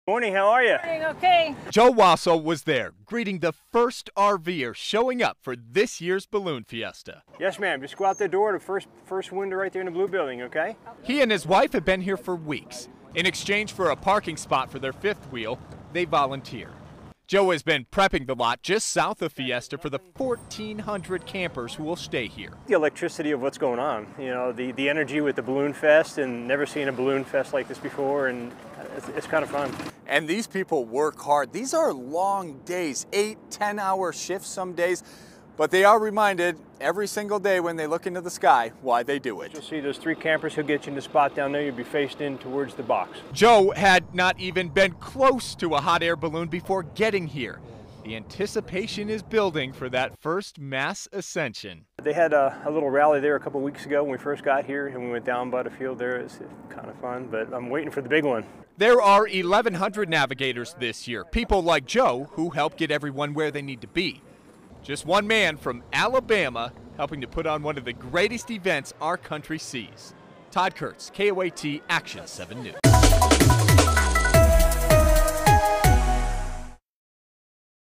Volunteers play major role in making Balloon Fiesta successful
Anchor Todd Kurtz shows us this huge event doesn't happen without an enormous effort from people behind the scenes.